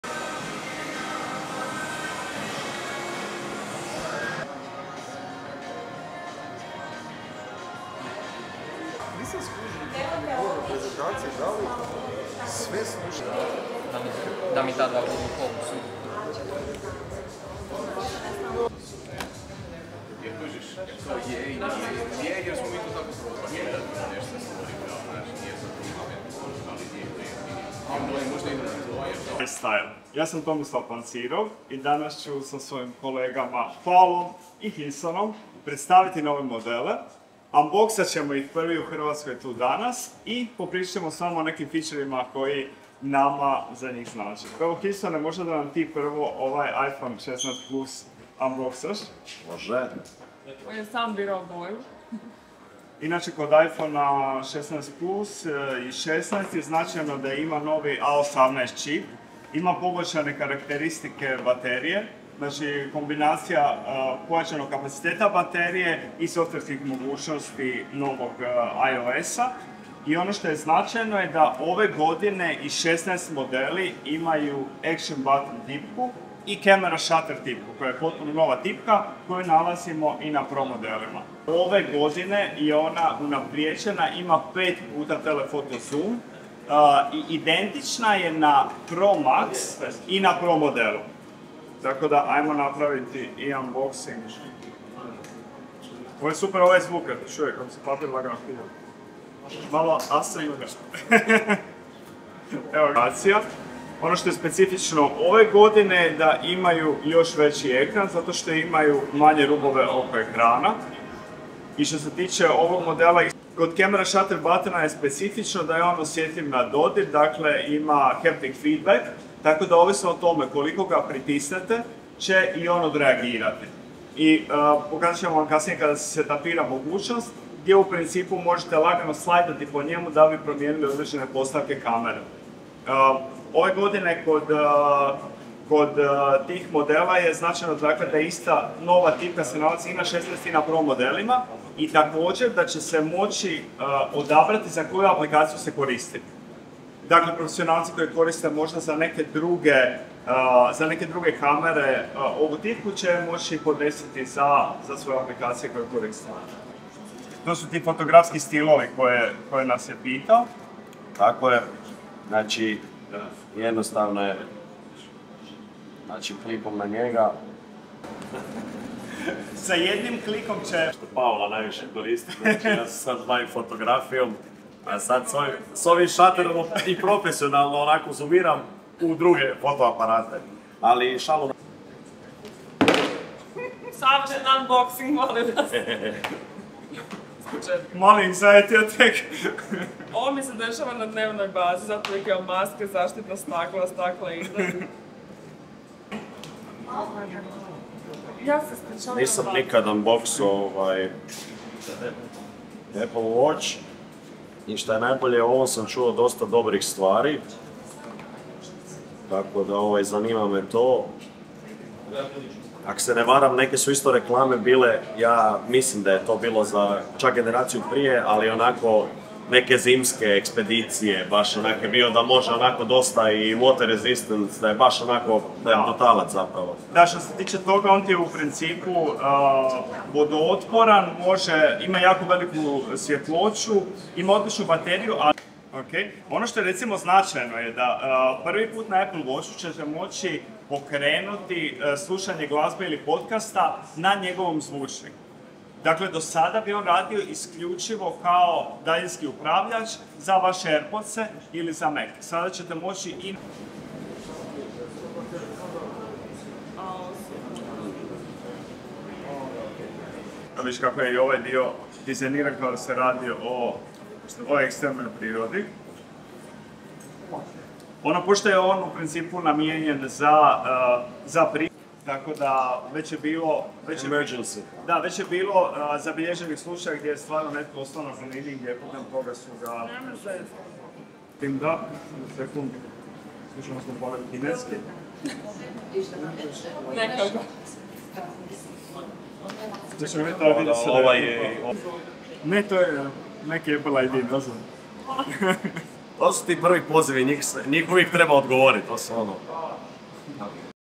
Novi iPhone 16 stigao je u Hrvatsku
Hiljson Mandela, Tomislav Pancirov I Paula Sikirić predstavili su u iSTYLE trgovini u Arena centru nove modele Iphonea 16.